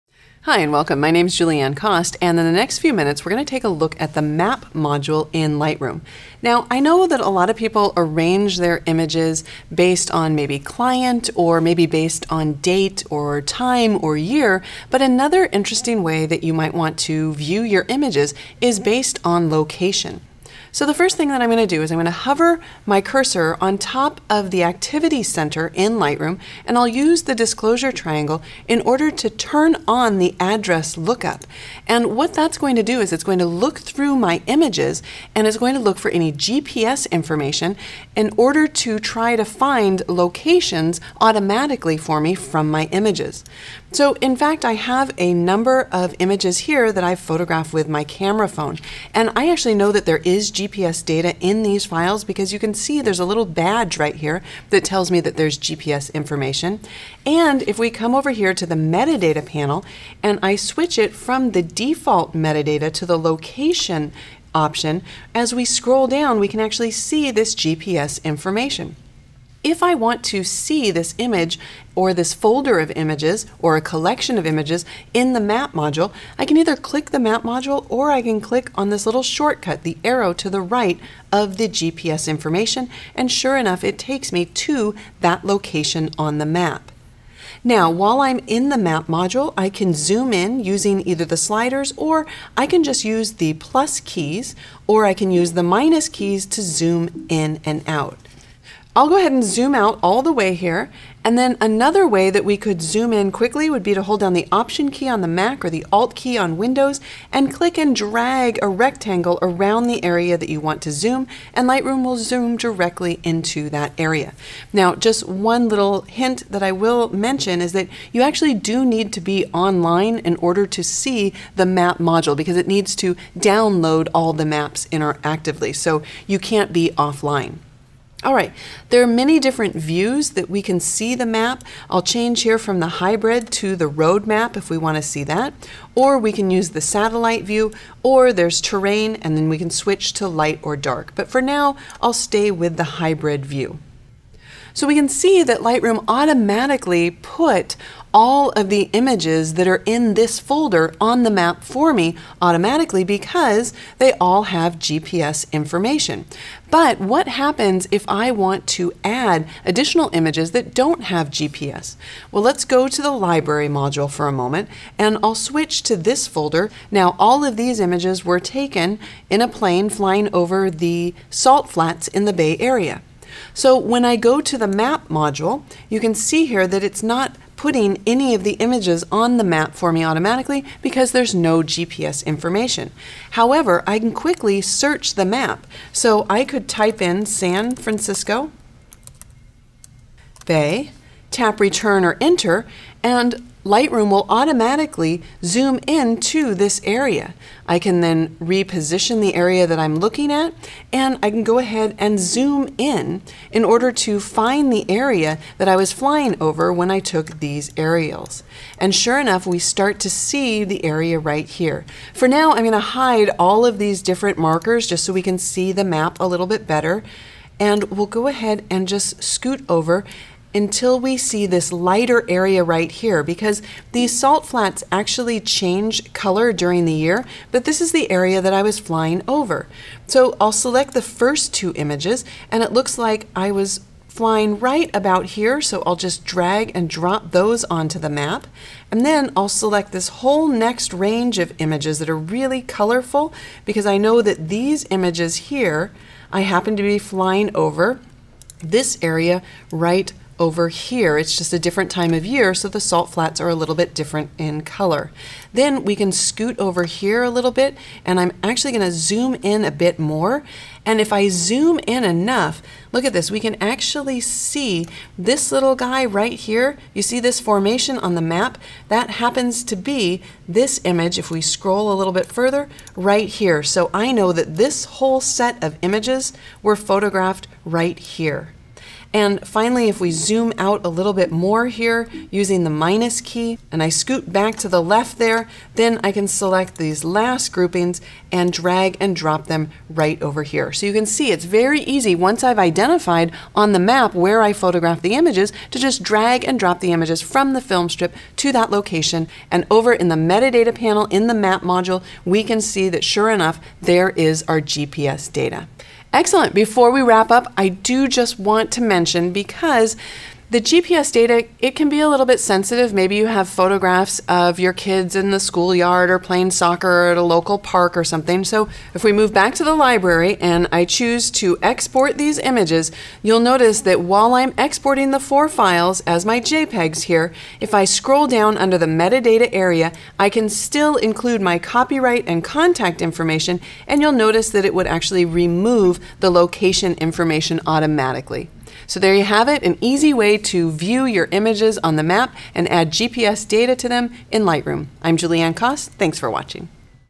Lightroom CC - Viewing Images on a Map | Adobe Lightroom
*The product featured in this video is Lightroom Classic CC, previously known as Lightroom CC.

Learn how to use the Map module in Lightroom to view images based on location information.